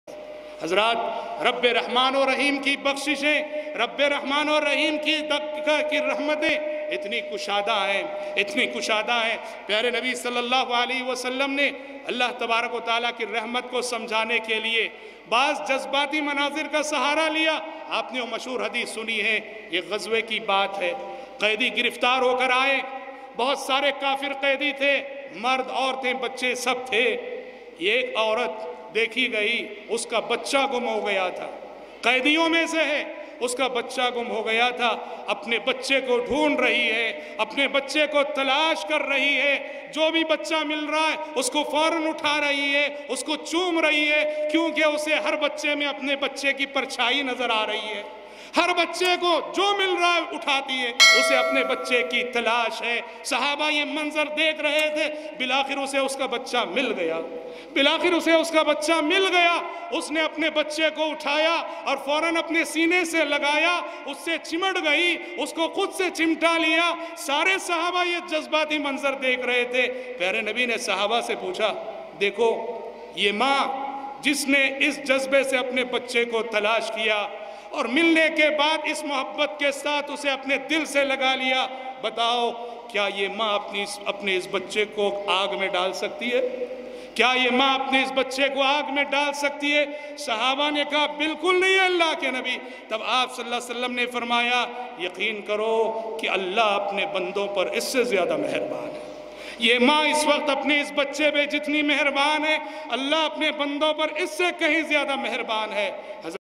Allah apne bando per ek maa se zyada meharban hai | Shaykh Abdul Azeem Madani
This video clips is taken from Allah ki Rehmat conference by Shaykh Abdul Azeem Madani